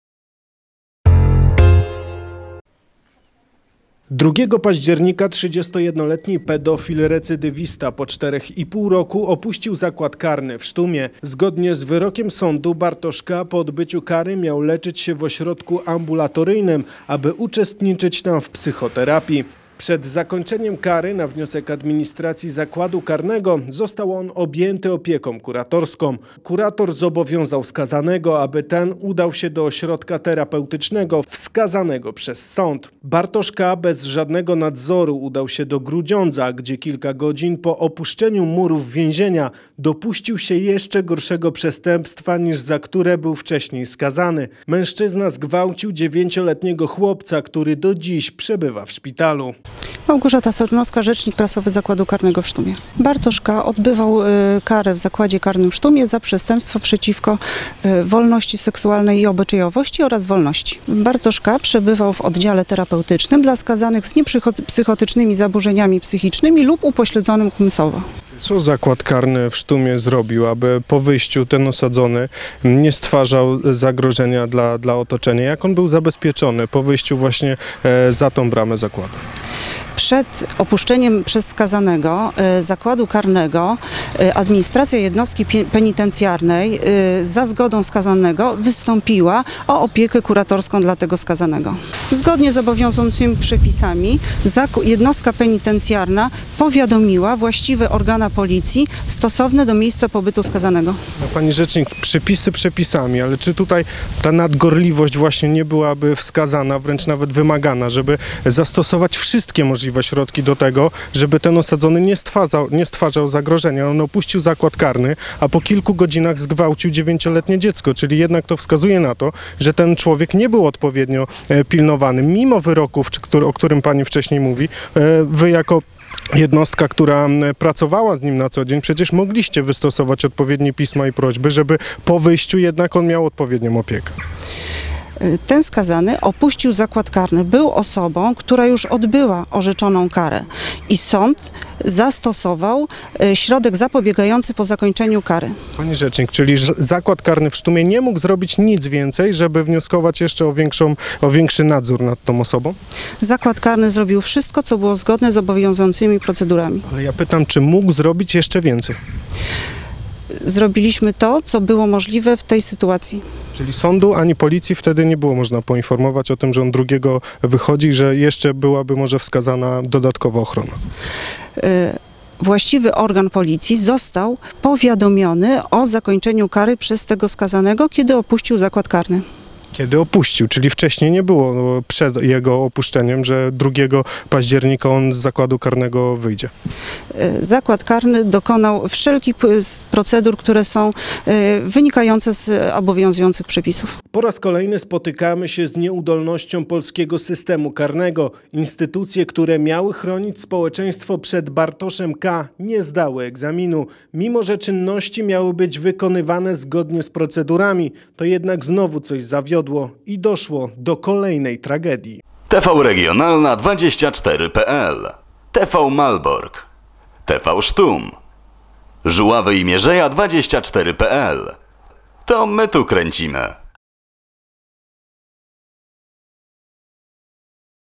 Pedofil opuścił Zakład Karny w Sztumie, po czym zgwałcił 9- letniego chłopca – 05.10.2015
2 października 31 – letni pedofil recydywista po 4,5 roku  opuścił zakład karny w Sztumie. Zgodnie z wyrokiem sądu Bartosz K. po odbyciu kary miał leczyć się w ośrodku ambulatoryjnym, aby uczestniczyć tam w psychoterapii.